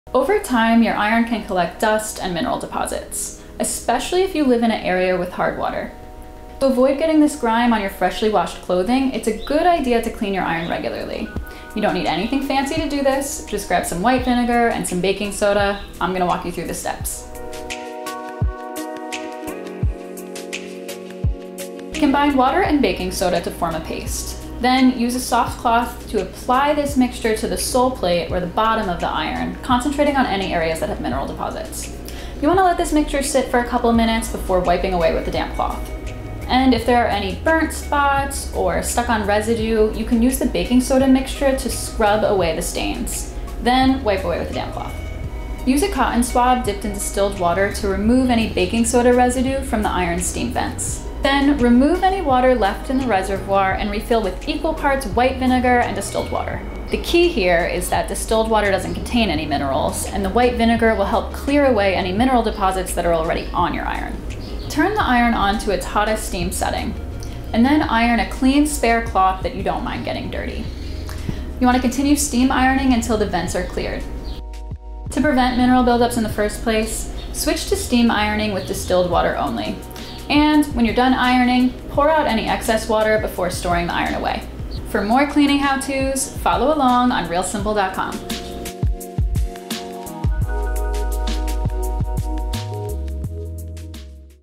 How to Clean an Iron (4 Easy Steps) | Spotless | Real Simple
Over time your iron can develop caked on mineral spots and burn marks, but with 4 easy steps you can have your iron looking good as new.

00:00 Introduction
00:22 Combine water and baking soda
00:25 Use a soft cloth to apply paste to iron
00:35 Scrub away burnt spots
00:46 Use a cotton swab with distilled water to remove residue
00:54 Refill resevoir with distilled water and white vinegar
01:04 Turn your iron on to the hottest setting
01:07 Iron a heated spare cloth

How to Clean an Iron in 4 Easy Steps | Spotless | Real Simple